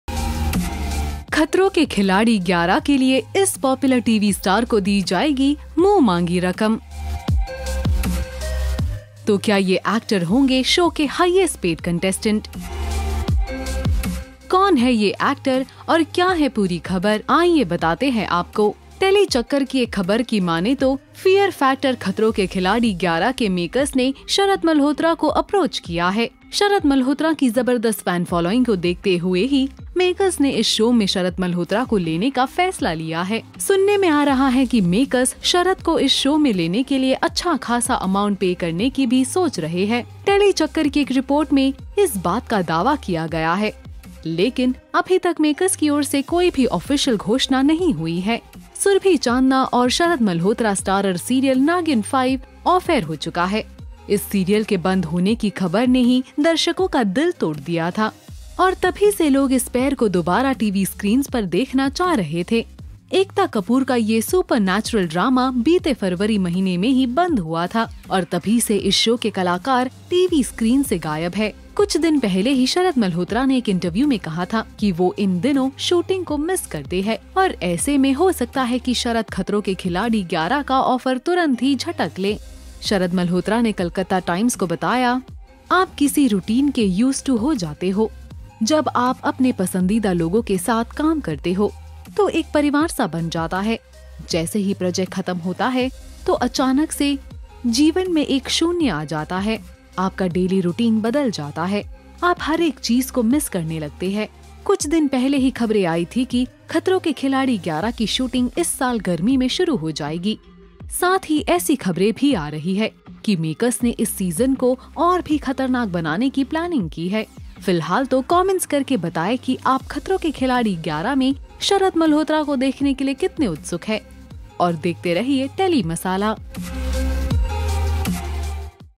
This TV Star To Be The Highest Paid Contestant For Khatron Ke Khiladi 11?
Watch the video as This TV Star might get paid the highest to participate in Khatron Ke Khiladi Season 11.